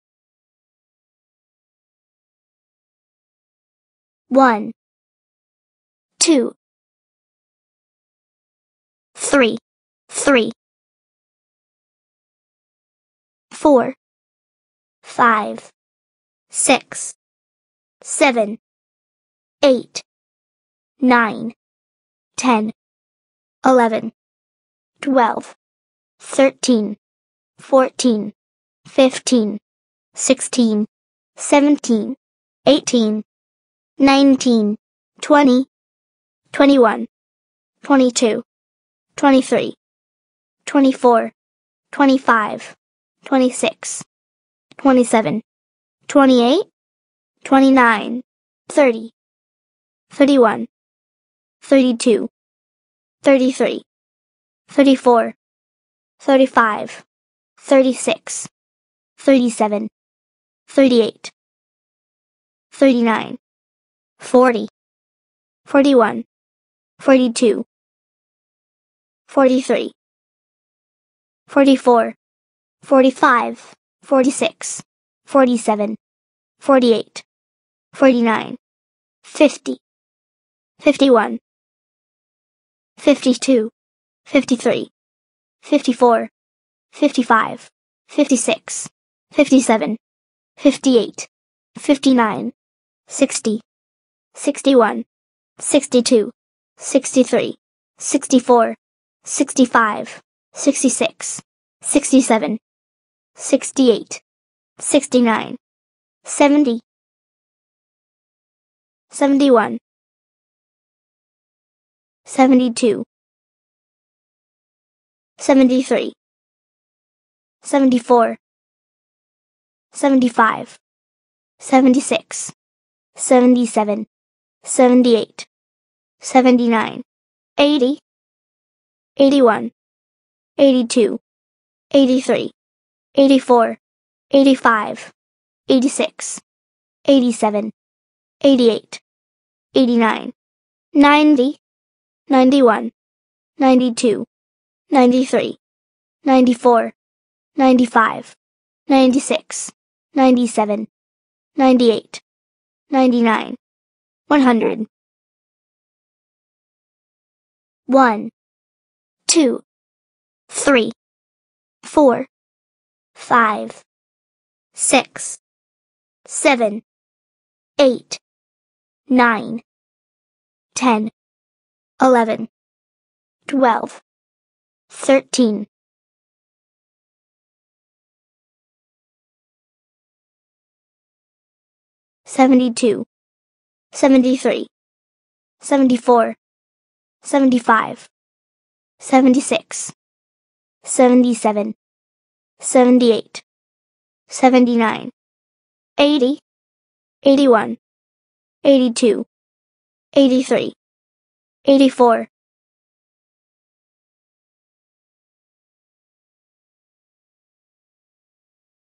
Numbers Flashcards 1 to 100 Preschool Learning
123 For Youngsters begins fabricating Numbers' shapes in kids' sub-cognizant psyche in early ages, so the data is reviewed when its instructed in schools. It is the most fit numbers cheat sheets application to be utilized in all degrees of kindergarten classes too.